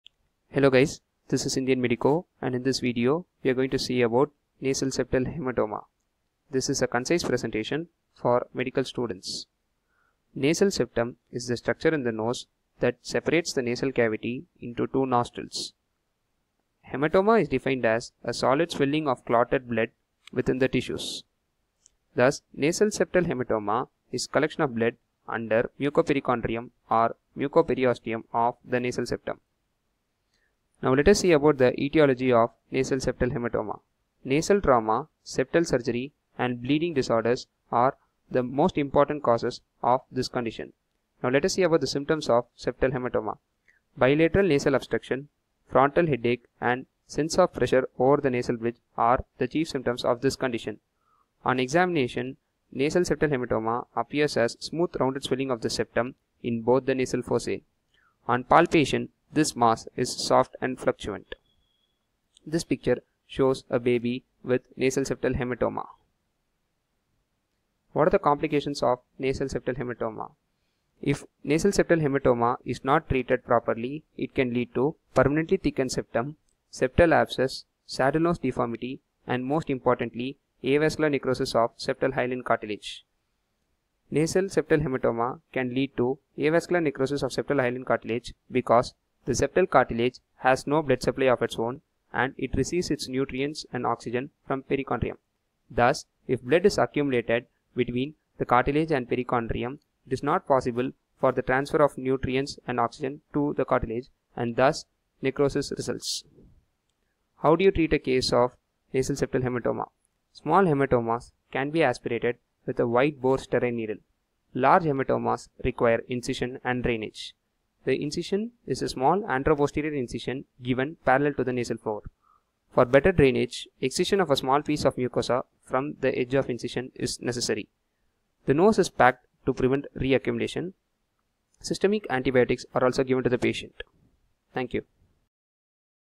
Nasal Septal Haematoma - ENT
Hey guys, this is Indian Medico. In this video, we are going to see about Nasal Septal Haematoma. This is a concise presentation for medical students (especially from India). I hope that this presentation helps you to understand the topic as well as score well in your exams. I welcome your feedback. If you don't like this video, hit the dislike button and comment your suggestions please.